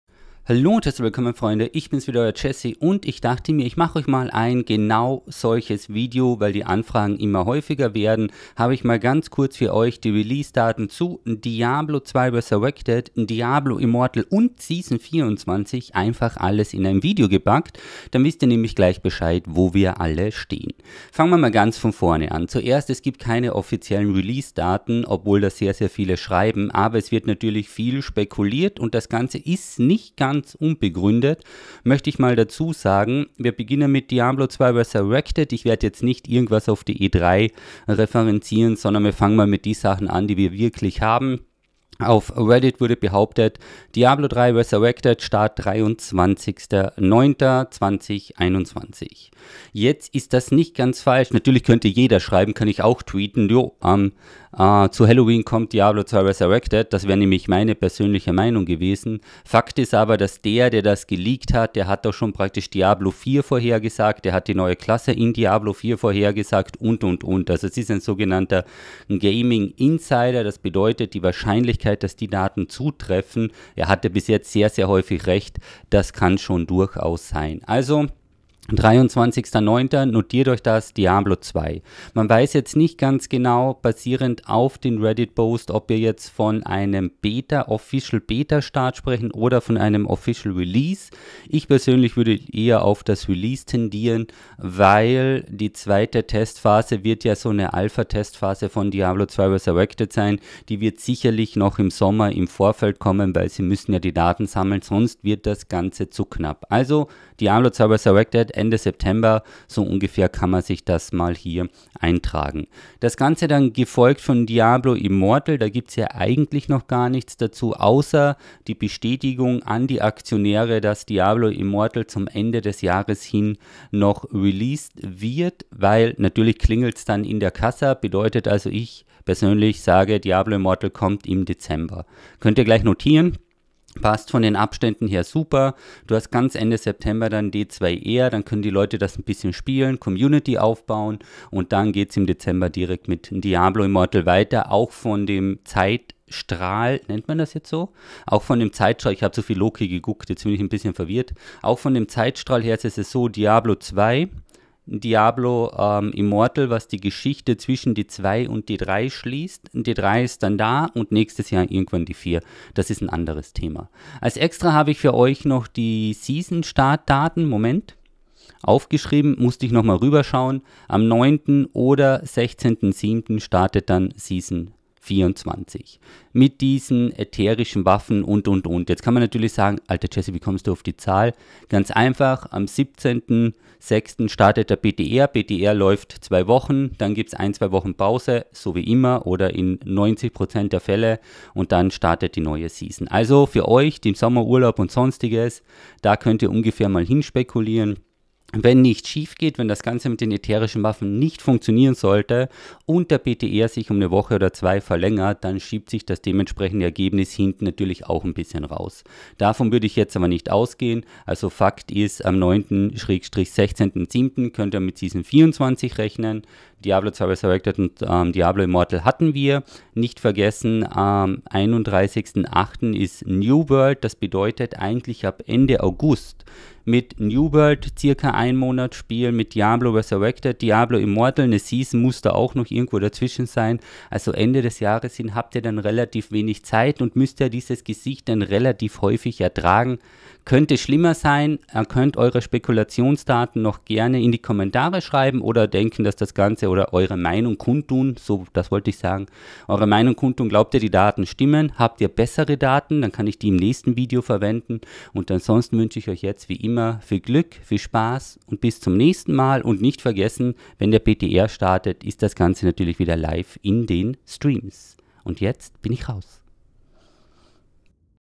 Alle Release Daten zu D2R +DI +Season 24 im Überblick
Ihr braucht noch den Release für Diablo 2 Resurrected um euren Urlaub einzutragen und wollt auch wissen wann Diablo Immortal kommt? Wir haben für euch alle Leaks & Spekulationen zusammengetragen und sagen Euch auch noch wann die Season 24 aus Diablo 3 startet.